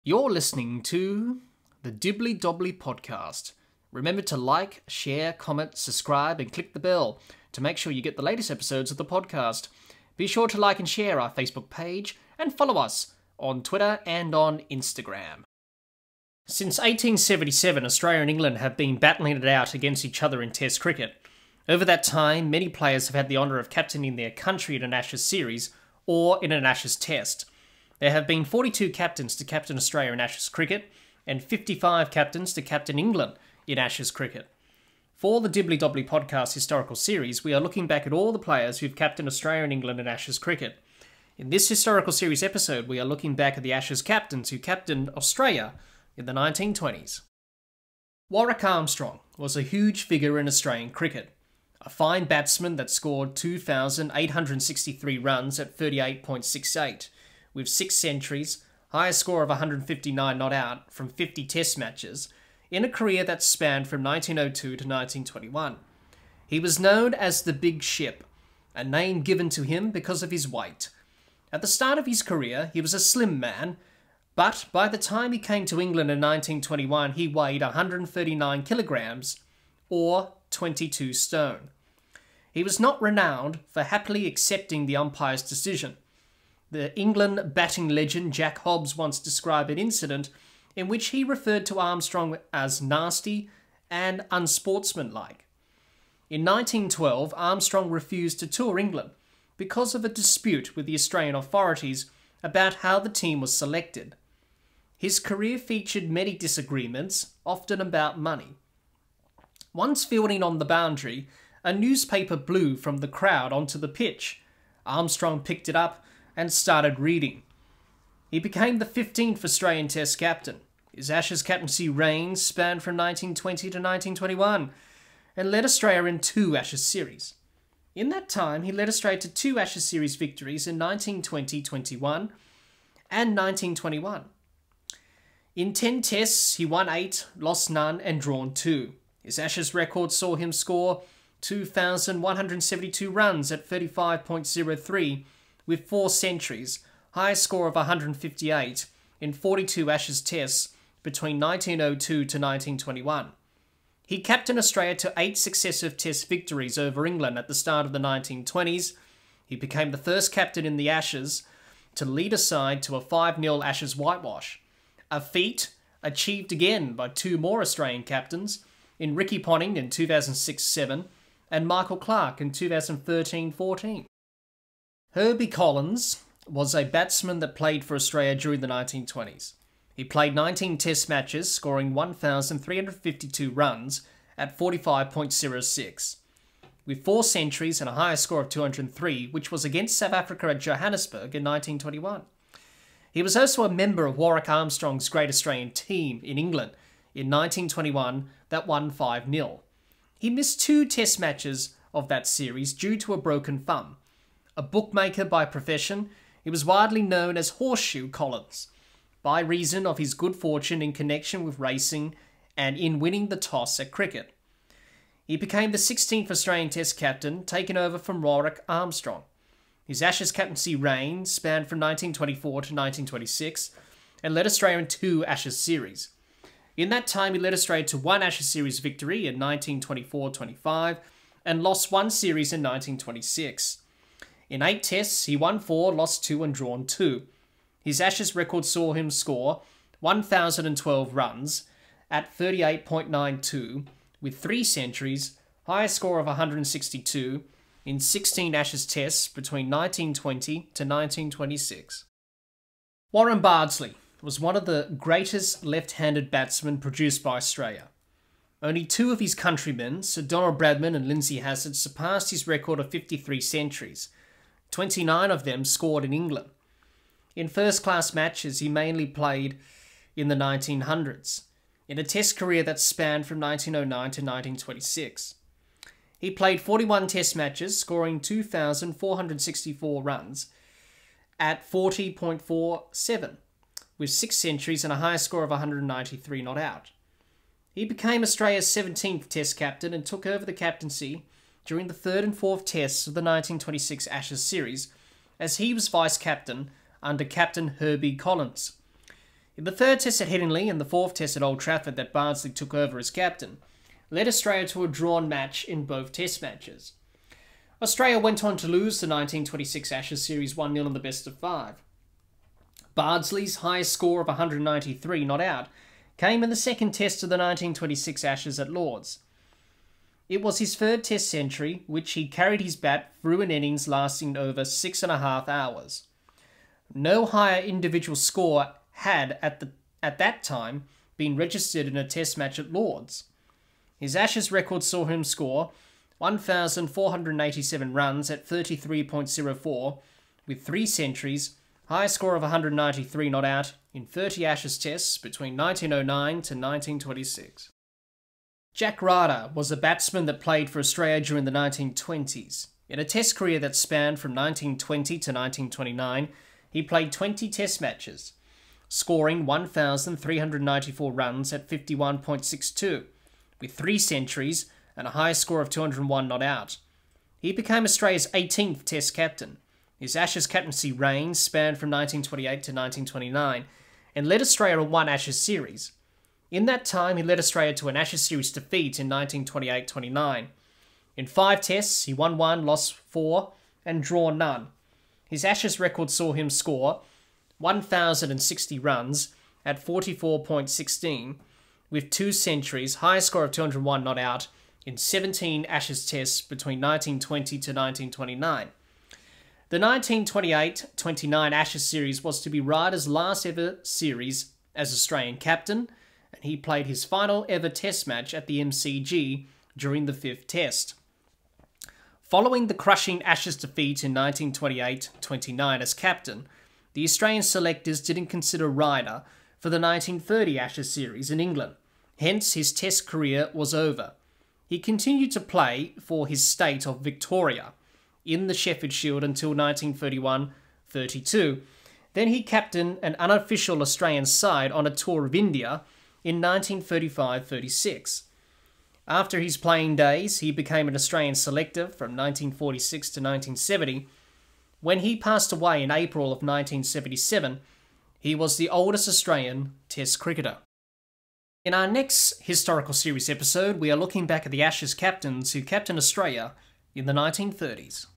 Australia | Ashes Captains | 1920’s | Historical Series
In this Historical Series episode we are looking back at the Ashes captains who captained Australia in the 1920’s.

Since 1877 Australia and England have been battling it out against each other in test cricket. Over that time many players have had the honour of captaining their country in an Ashes series or in an Ashes test. There have been 42 captains to captain Australia in Ashes cricket and 55 captains to captain England in Ashes cricket. For the Dibbly Dobbly Podcast Historical Series we are looking back at all the players who’ve captained Australia and England in Ashes cricket.

0:00 Intro
0:48 Warrick Armstrong
2:55 Herbie Collins
4:13 Warren Bardsley
5:48 Jack Ryder
7:31 Outro

Podcast Services